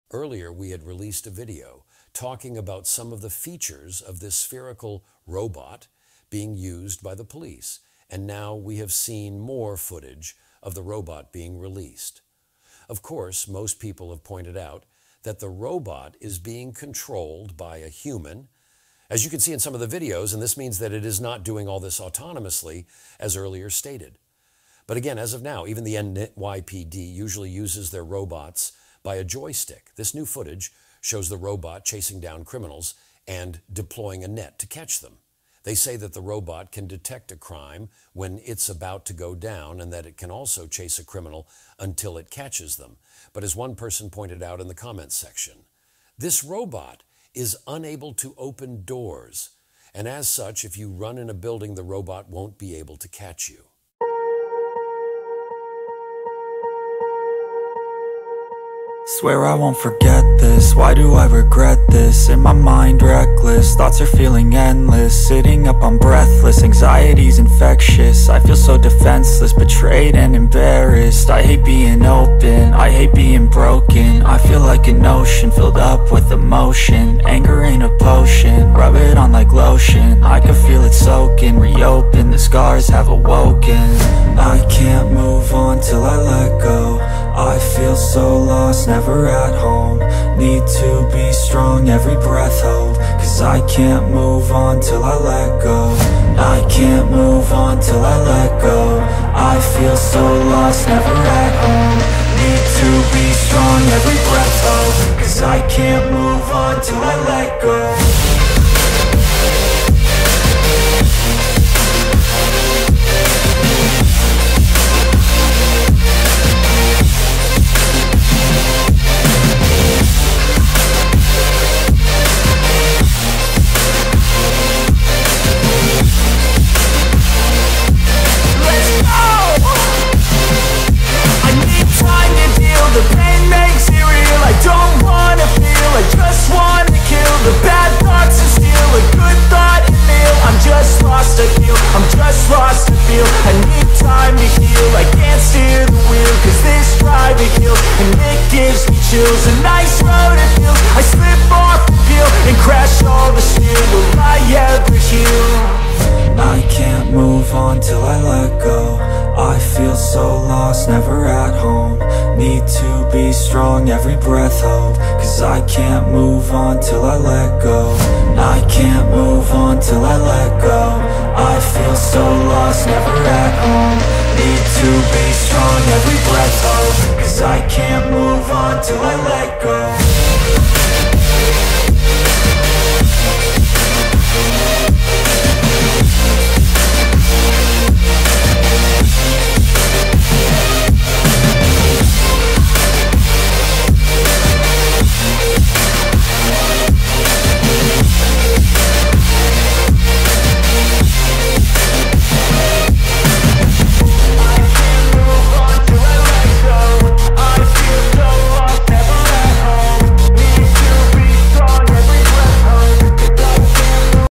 China Spherical Police Robot is Remote Controlled: Watch Demo of Cathing Criminals
China Spherical Police Robot is Remote Controlled: Watch Demo of Cathing Criminals. Earlier on we released a video showing the Features of the Robot Policeman Patrolling Streets of China,. This Sphere robot Police is Packed with Advanced Technologies and can go toe to toe with criminals who are trying to flee from the Robot Sphere/